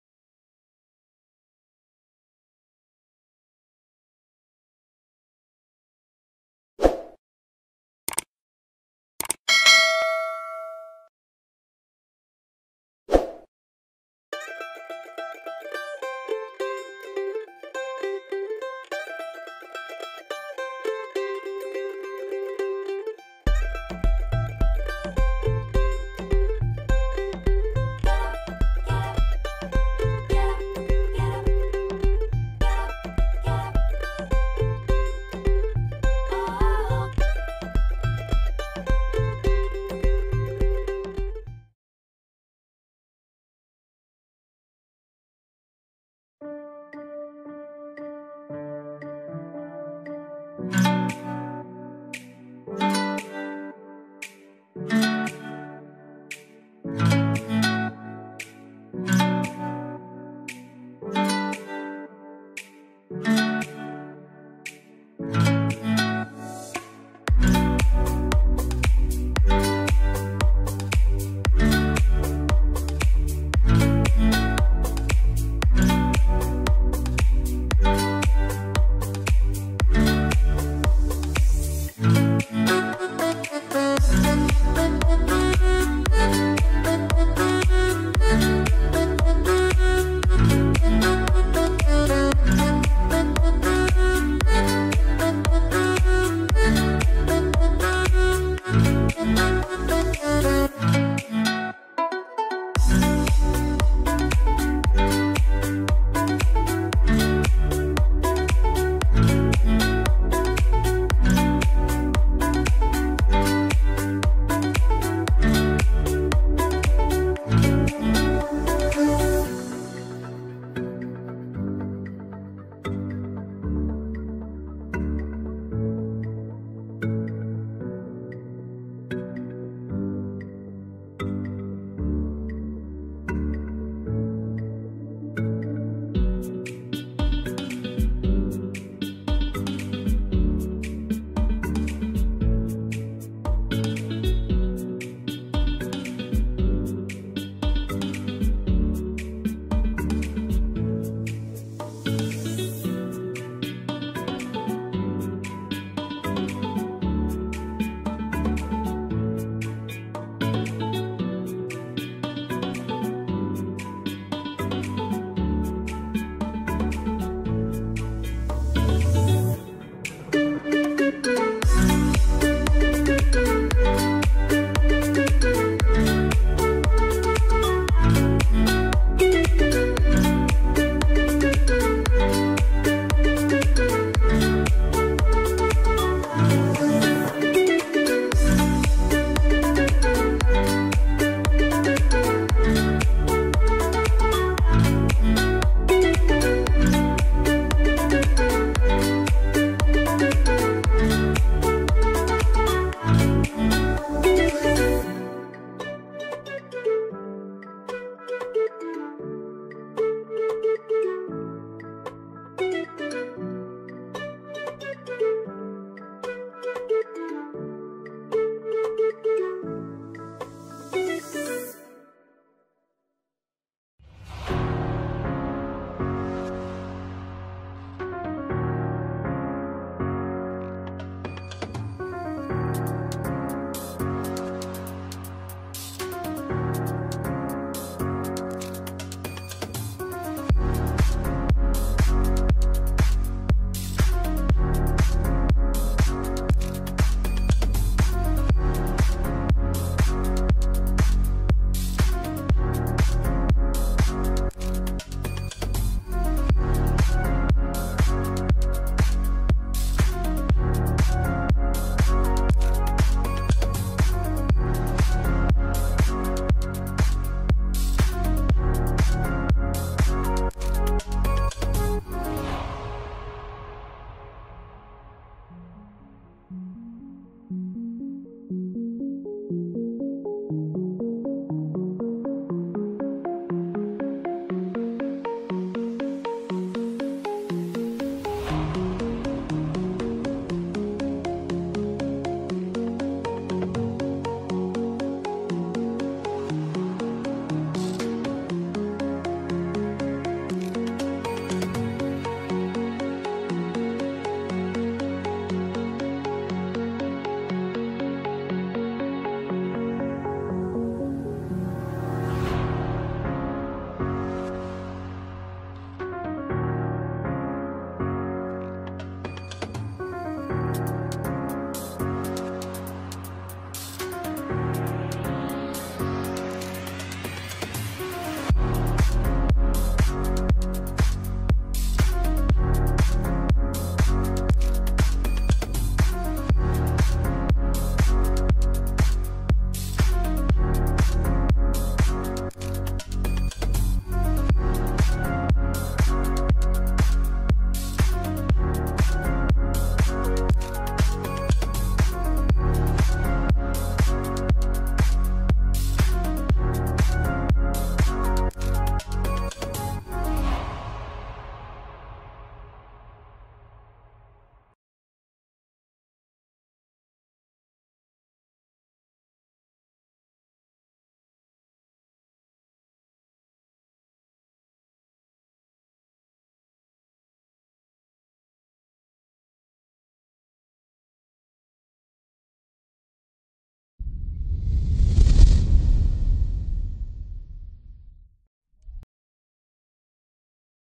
Beach Preset | using Lightroom mobile | Lr Just Effects
In this video , I will show you how to edit beach photography presets using lightroom mobile.

Watch the full tutorial👍🏻

                     justeffects321

                     Just Effects2021